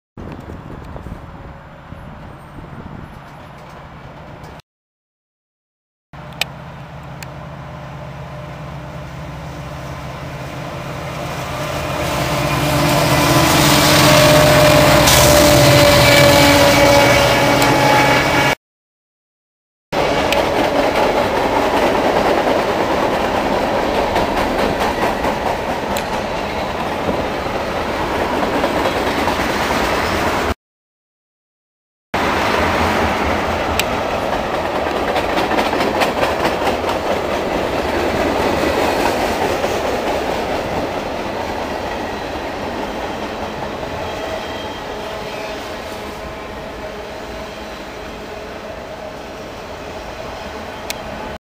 CN Freight at Aldershot March 3 2010
CN Freight at Aldershot March 3 2010 with Illinois Central SD70